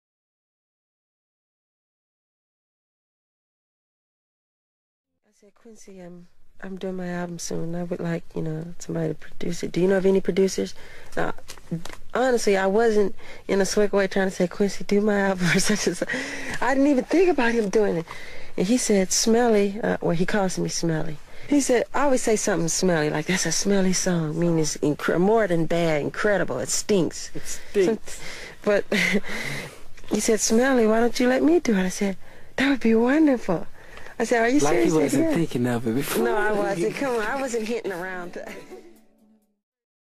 Michael Jackson Rare Talking about Quincy Jones
Michael Jackson Rare Talking about  Quincy Jones//In his own words
MichaelJackson.gr